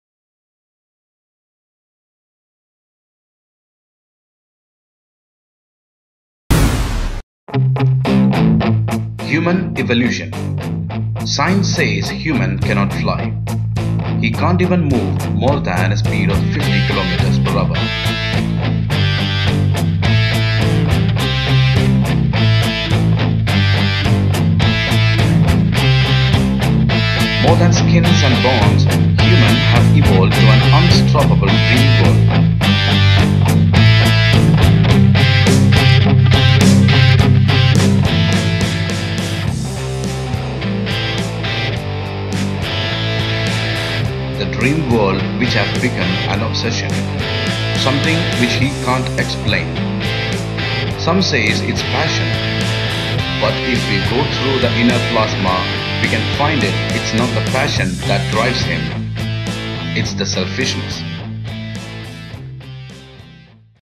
Video Montage - The Human Evolution | Legacies Media
This video montage was ideated and visualised on a weekend when we where traveled through the city life, The modernised city life which  we are excited about is turned to be something else when it was shown to us, look where human evolution had took us.

Voice : Chandramauly Vijayan

by go1dfish

Attribution